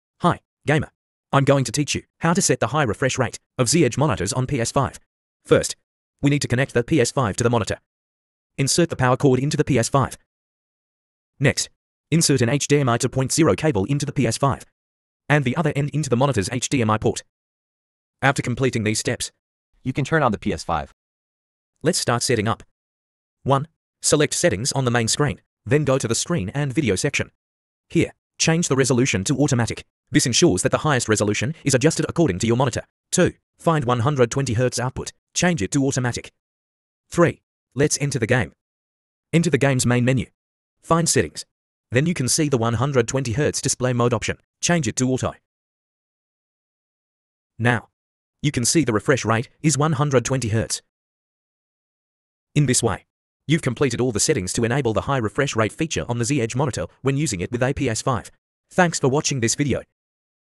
How to set a high refresh rate for PS5 on a Z - EDGE monitor🎮️
Still annoyed that you can't get 120Hz when playing games after connecting your PS5 to a monitor? This video will clearly show you how to set the refresh rate of PS5 to 120Hz on a Z - EDGE monitor, and make your gaming smoother.